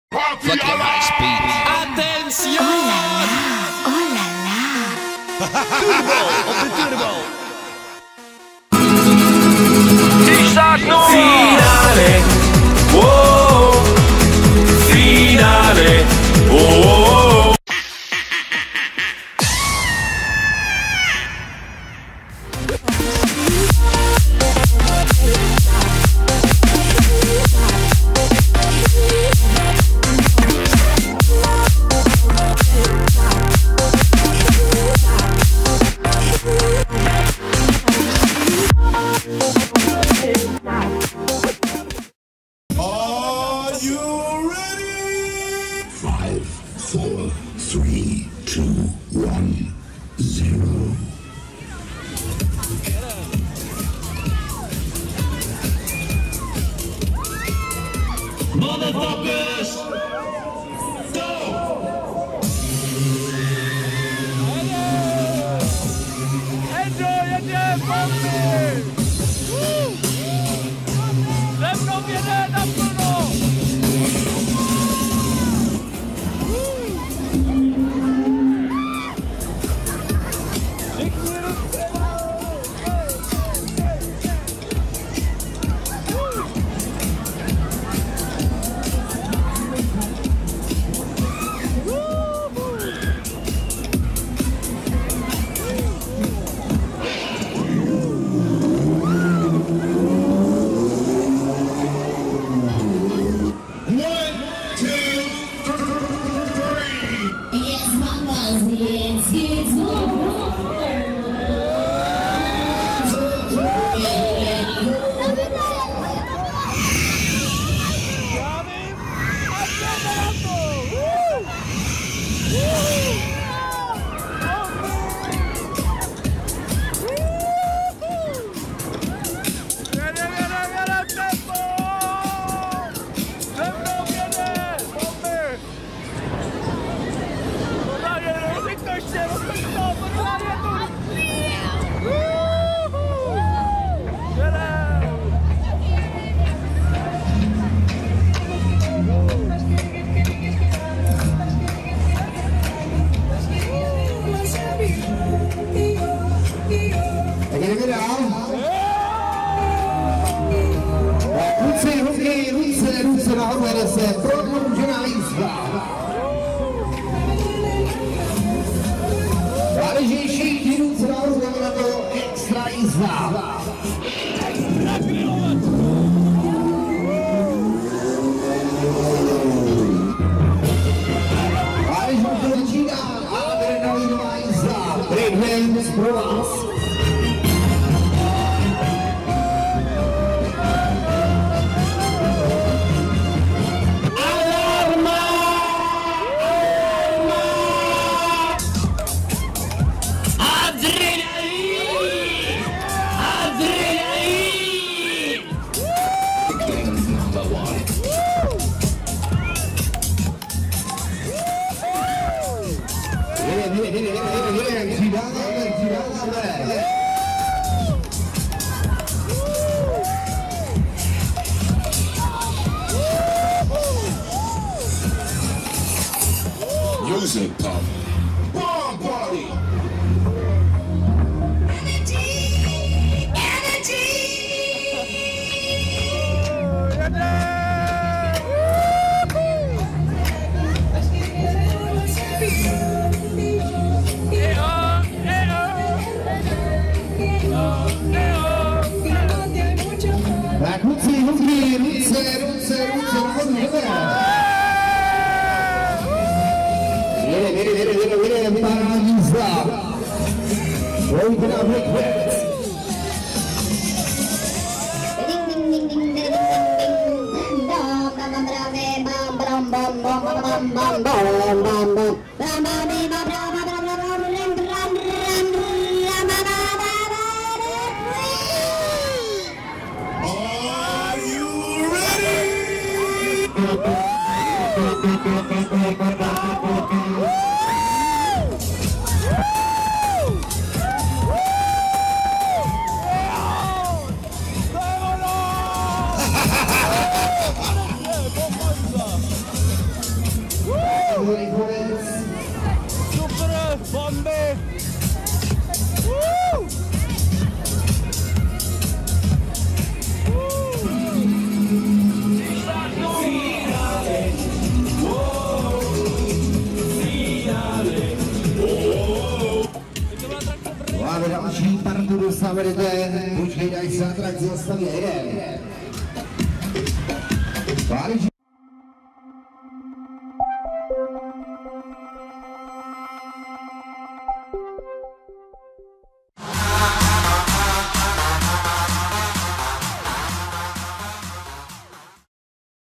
Břevnovské posvícení 2017 - ATRAKCE BREAK DANCE ZA JÍZDY / ATTRACTION BREAK DANCE ON-RIDE
Atrakce BREAK DANCE za jízdy na Břevnovském posvícení 2017 v Praze na Břevnově. Jízda pro jednu osobu 80 Kč (80 CZK). Natáčeno v sobotu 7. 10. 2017. Lunapark Helfer.
LUNAPARK HELFER - NEJVĚTŠÍ LUNAPARK V PRAZE.
Břevnovské posvícení 2017 v Praze na Břevnově se koná od čtvrtka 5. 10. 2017 do neděle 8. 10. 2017.